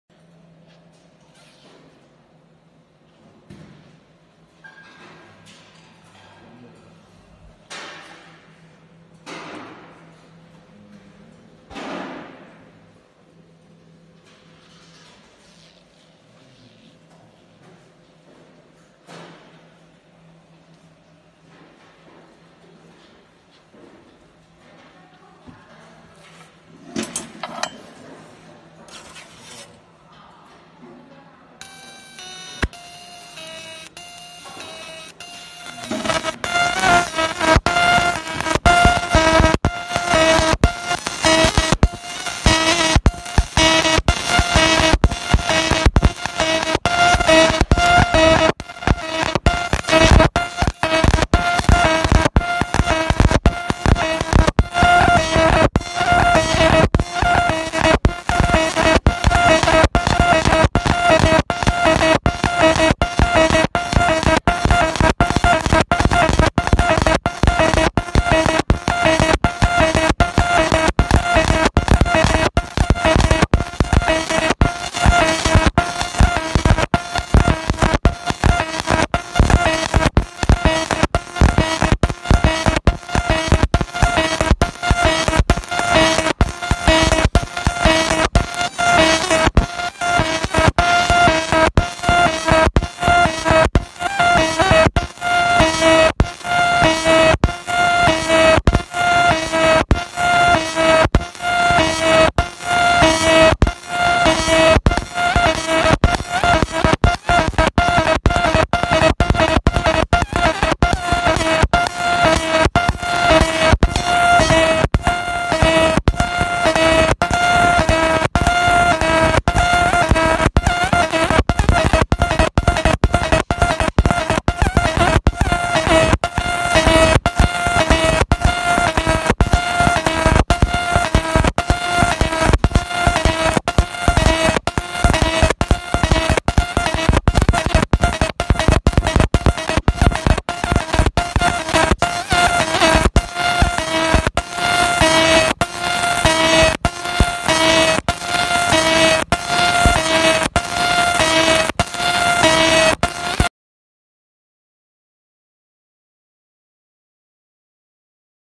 2020 Best 3D Fiber Laser Engraving Machine for Engraving Steel Test.
Looking for an affordable laser engraving machine for steel, or other metal? Check out 2020 best laser engraver buying guide to engrave steel and other metals. 3D Fiber Laser Engraving Machine is suitable for surface engraving, it can engrave the effect you are satisfied with.

We will keep updating new 3D laser engraver videos.